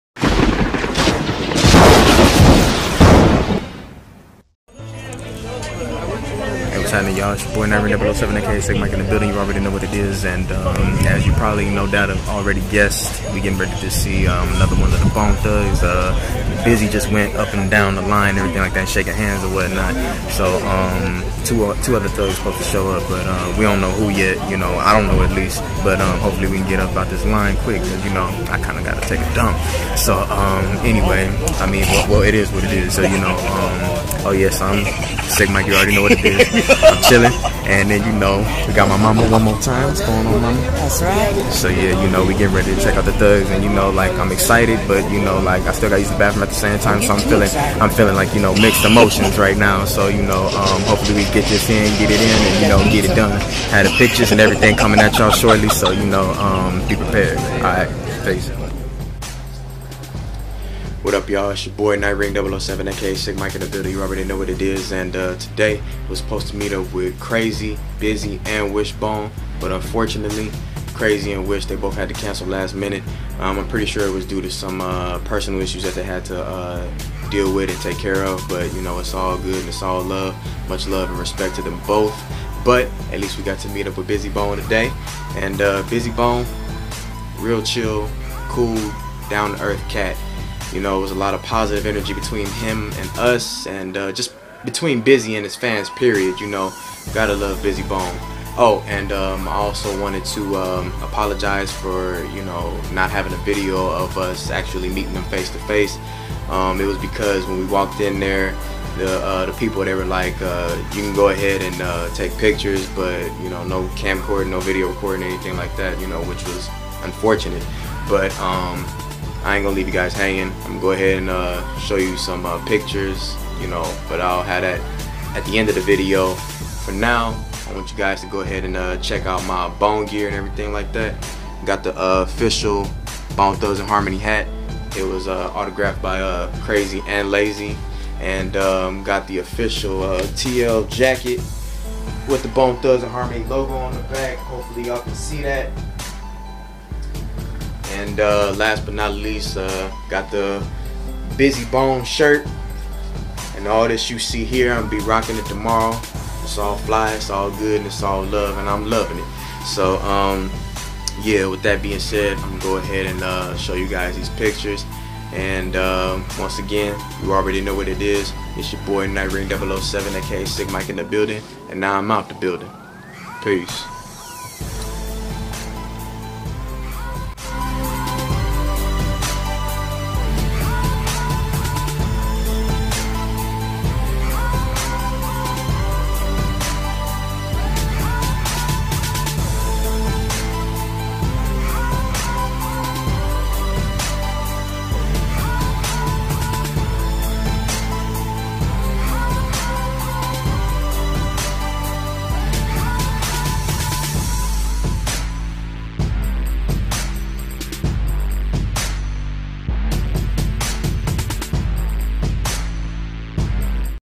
Meeting Bizzy Bone (Second Time)
Me meetin' up with the legendary Bizzy Bone at the Official TL Store in Downtown L.A. Had a mad good time! :)